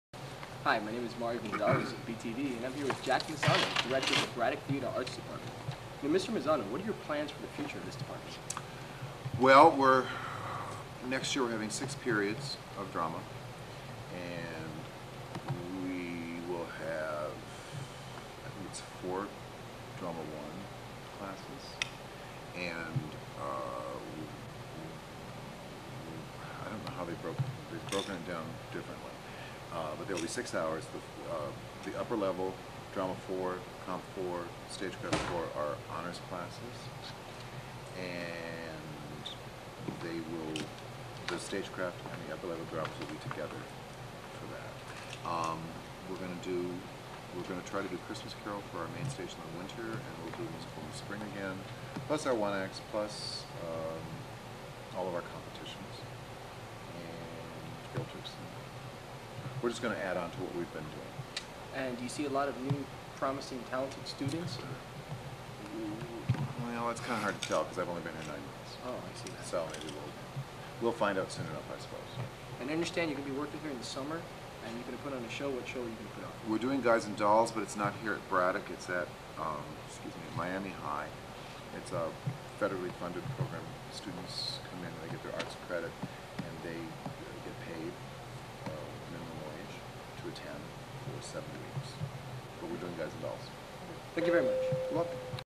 BTV 8 Jack Mezzano Interview (1998)
A 1998 interview with Braddock High School Drama Teacher Jack Mezzano by Mario Xavier.